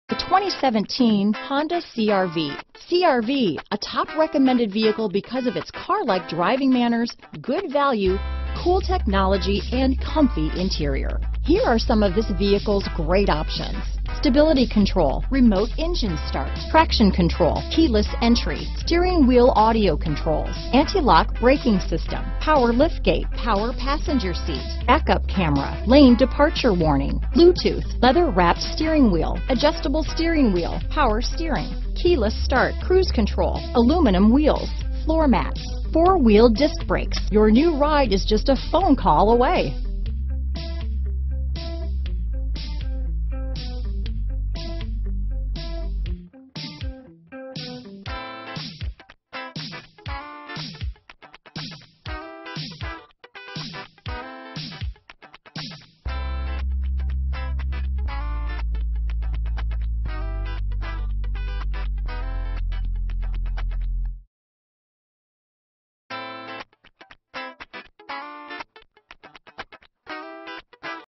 2017 Honda CR-V Marietta, Atlanta, Roswell, Woodstock, Kennesaw, GA 489863
2017 Honda CR-V

New Arrival! -Great Gas Mileage- *Backup Camera* *Bluetooth* *Heated Front Seats* *Keyless Start* *Turbocharged Engine* *Sunroof/Moonroof* This 2017 Honda CR-V EX-L is Basque Red Pearl Ii/Gray with a Gray interior. This Honda CR-V EX-L comes with great features including: Backup Camera, Bluetooth, Leather Seats, Heated Front Seats, Sunroof / Moonroof, Satellite Radio, Blind Spot Assist, Lane Assist, Remote Engine Start, and Turbocharged Engine . It is rated at 34.0 MPG on the highway which is fantastic! Buy with confidence knowing Ed Voyles Honda has been exceeding customer expectations for many years and will always provide customers with a great value!